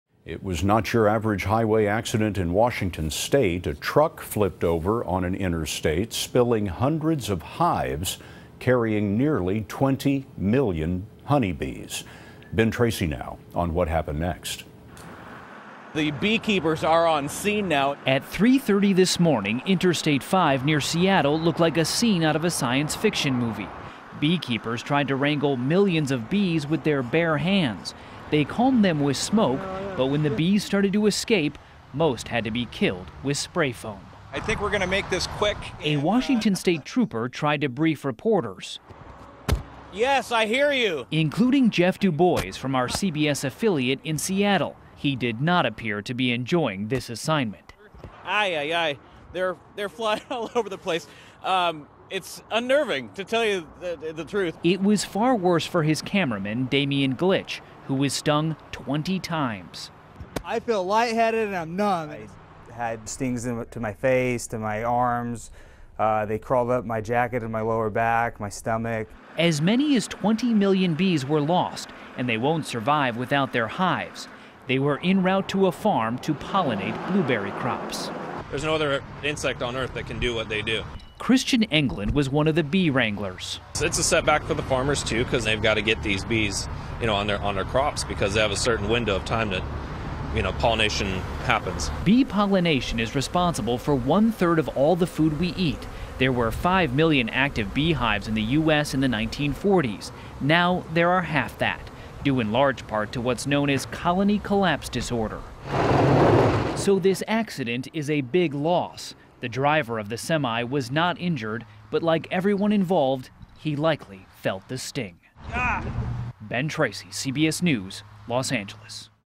Millions of bees escape near Seattle
A truck carrying more than 400 beehives overturned at Interstate 5 near Seattle. Beekeepers tried to wrangle millions of bees with their bare hands while news crews tried to avoid getting stung. Ben Tracy reports.